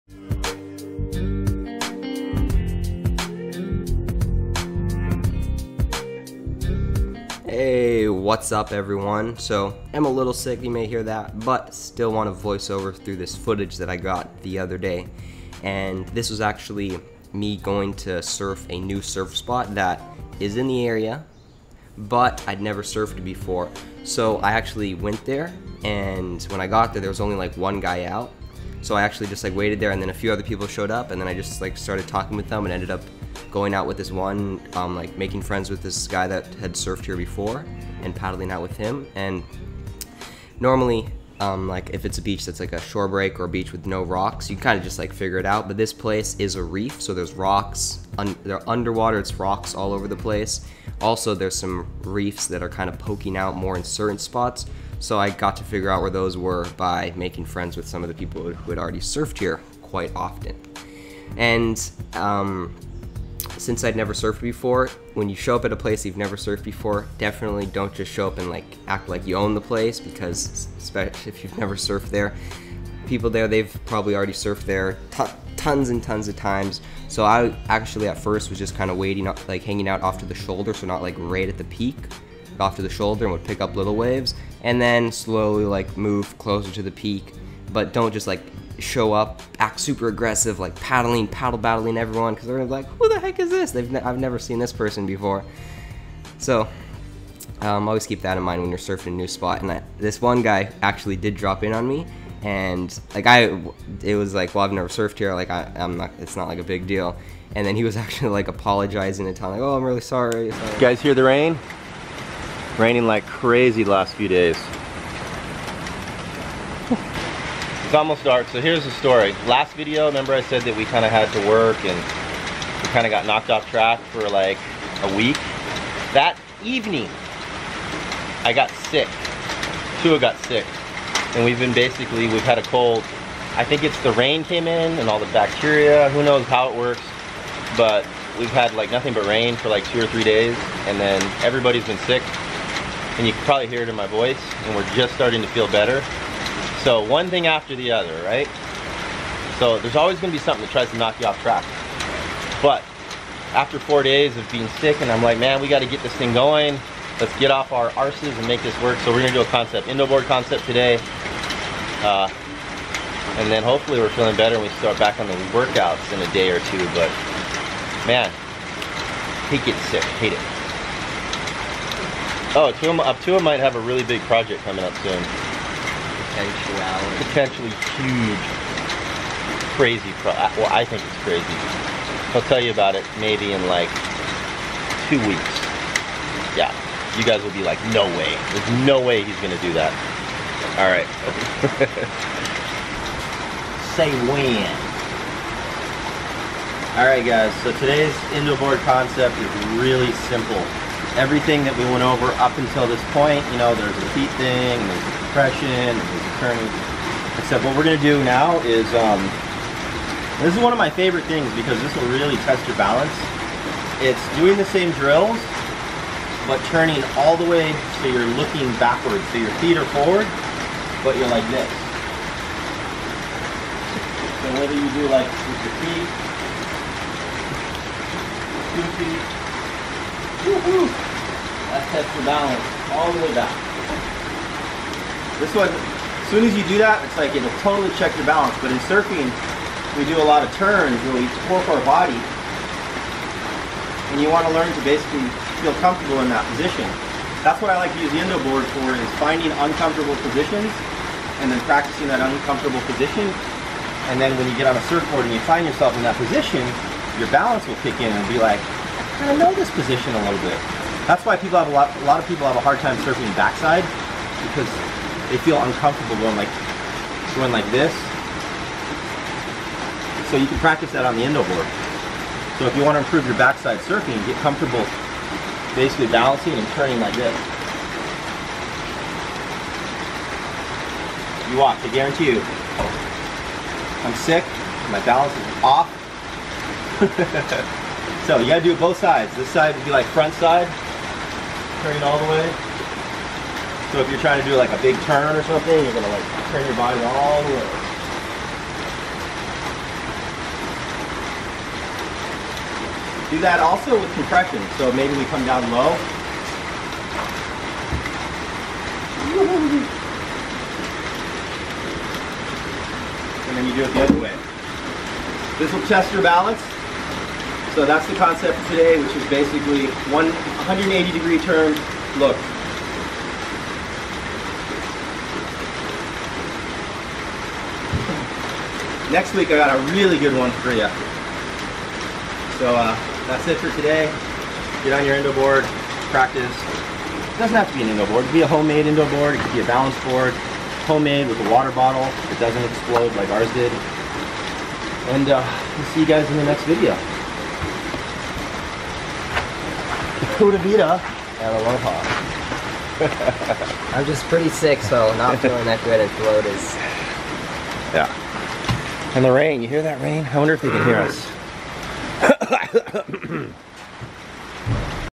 Surfing a New Spot & Twisting Balance Exercise - Ep 18
Back with a quick surf vlog and balance training concept.